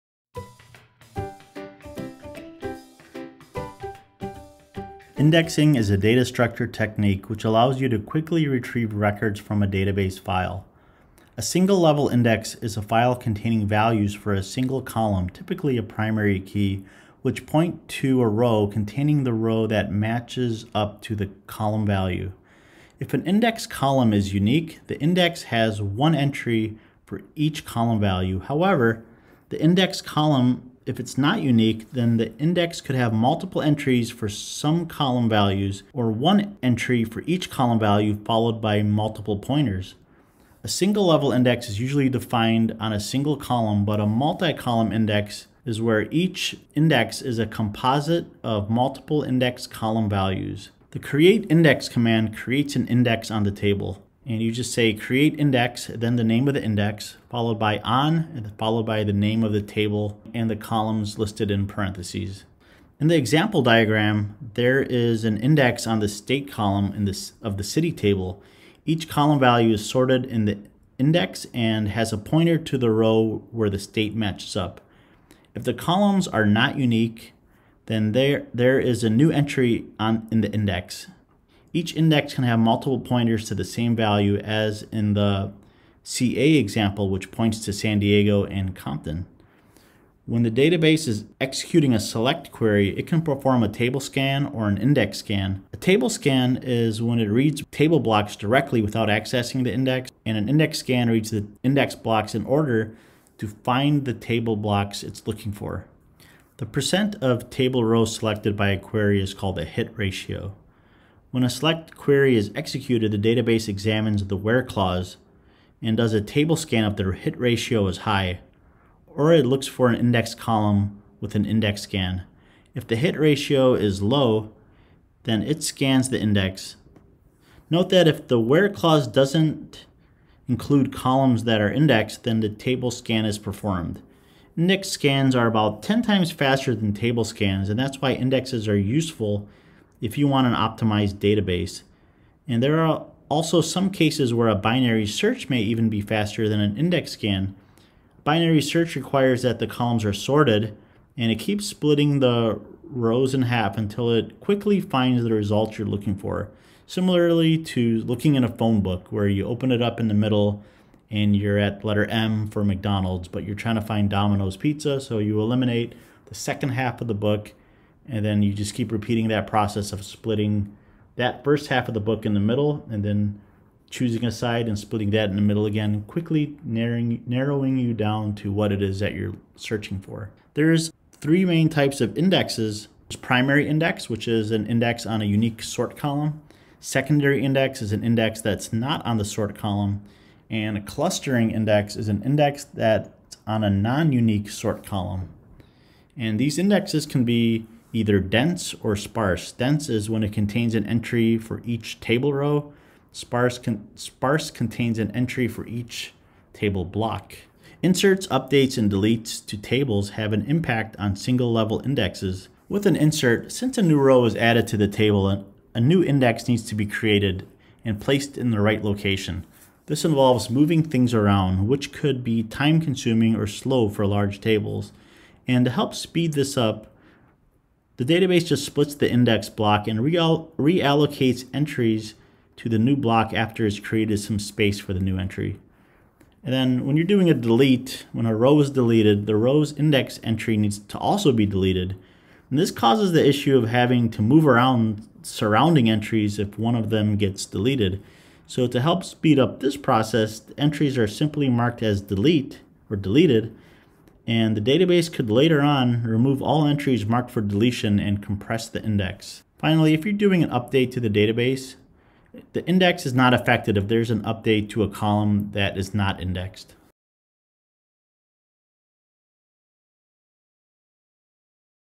Database Systems - Single Level Indexes - Primary Secondary Clustering Dense and Sparse Index
Indexing is a data structure technique which allows you to quickly retrieve records from a database file

A single-level index is a file containing values for a single column (typically a primary key), which point to a row containing the row that match up to the column value.

If an indexed column is unique, the index has one entry for each column value. However, if the indexed column is not unique, the index could have multiple entries for some column values, or one entry for each column value, followed by multiple pointers.

A single-level index is usually defined on a single column, but in a multi-column index, each index is a composite of multiple indexed column values.

The CREATE INDEX command creates an index on a table:

CREATE INDEX index_nameON table_name (column1, column2, ...);

For example:

CREATE INDEX IndexLastNameON Person (LastName);

In the example diagram, there is an index on the State column of the City table. Each column value is sorted in the index, and has a pointer to the row where the states match up. If the columns are not unique, then they are a new entry in the index. Each index can have multiple pointers for the same value, as in the CA example which points to San Diego and Compton.

When the database is executing a SELECT query, it can perform a table scan or an index scan
A table scan reads table blocks directly, without accessing an index.
An index scan reads index blocks in order to find the table blocks its looking for.

The % of table rows selected by a query is called the hit ratio. When a SELECT query is executed, the database examines the WHERE clause does a table scan if the hit ratio is high, or looks for an indexed column with an index scan, if the hit ratio is low it scans the index. Note that if the WHERE clause doesn’t include column that is indexed, then a table scan is performed.  Index scans are about 10x faster than table scans, and that is why indexes are useful if you wan an optimized database. There are also some cases where a binary search may be faster than an index scan. Binary search requires that a column be sorted, and it keeps splitting the rows in half, until it quickly finds the results it’s looking for. Similarly to a phone book, when you are looking for Dominos pizza, and you open the book in the middle and you’re at letter M for McDonalds, you can eliminate the 2nd half of the book and repeat the process for the first half.

Primary Index is an index on a unique sort column.
Secondary index is an index that is not on the sort column.
Clustering index is an index on a non-unique sort column.

These indexes can be one of two types:
Dense Index - contains an entry for each table row.
Sparse Index - contains an entry for each table block.

Inserts, updates, and deletes to tables have an impact on single-level indexes.
Insert – Since a new row is added to a table, a new index needs to be created and placed in the right location. This involves moving things around, which could be time-consuming or slow for large tables. To help speed this up, the database just splits the index block and reallocates entries to the new block after it created some space for the new entry.

Delete. When a row is deleted, the row's index entry needs to also be deleted. This also causes the issue of having to move around surrounding entries if one of them gets deleted, so to help speed this process up, the entries are simply marked as ‘deleted’. The database could later remove all entries marked for deletion and compress the index.

Update. An index is not affected if there is an update to a column that is not indexed.